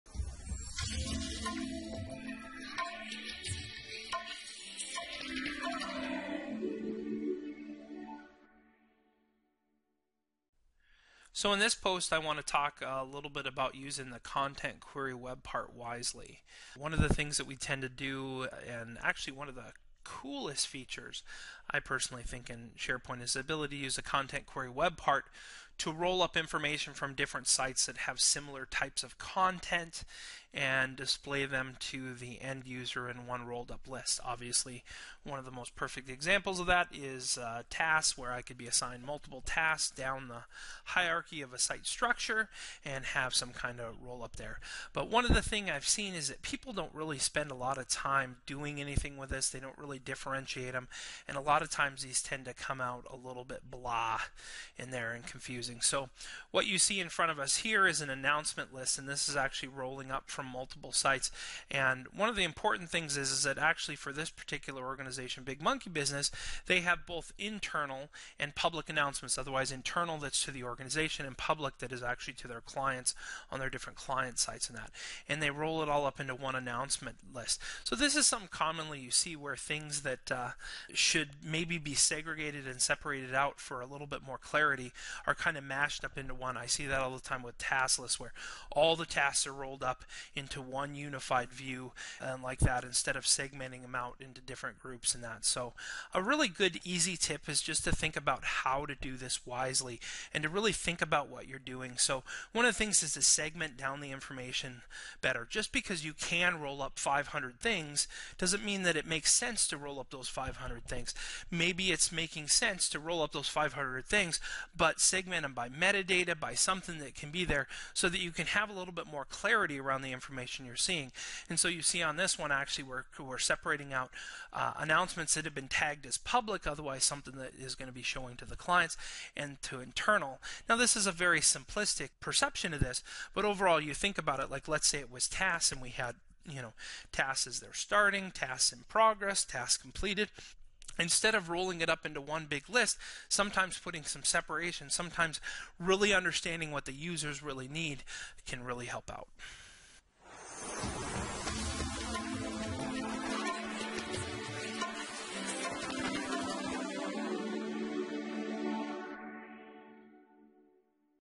101 SharePoint Usability Tips - # 40 Use CQWP roll-ups wisely
An easy way to display information in various forms is to use the content query web part ('CQWP'). When using the CQWP tool it is important to avoid bunching too much information into one area. Keep information manageable so it is easy to read and understand.